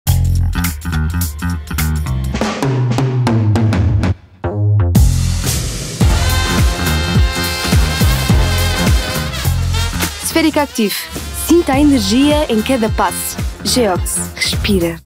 Geox | Spherica Actif™ commercial | SS23 - PT
Descubra Spherica™ Actif, o novo sapato Geox que amortece o impacto da caminhada e lhe proporciona mais energia em cada passo, graças ao inovador Zero Shock System com Energy Effect.